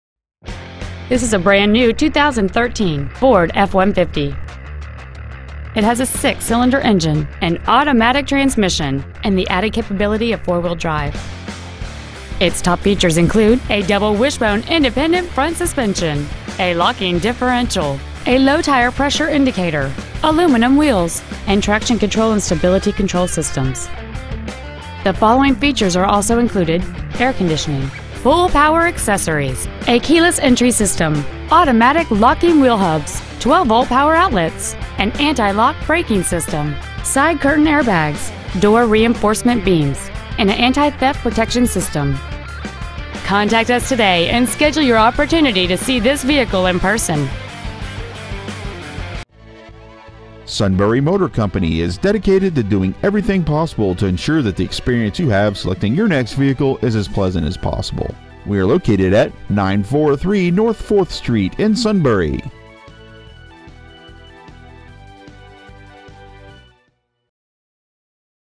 2013 FORD F-150 Sunbury PA
Year : 2013
 Make : FORD
 Model : F-150
 Engine : 3.5L V6 24V GDI DOHC TWIN TURBO
 Trans . : 6-SPEED AUTOMATIC
 Exterior : WHITE PLATINUM

 Stock : P909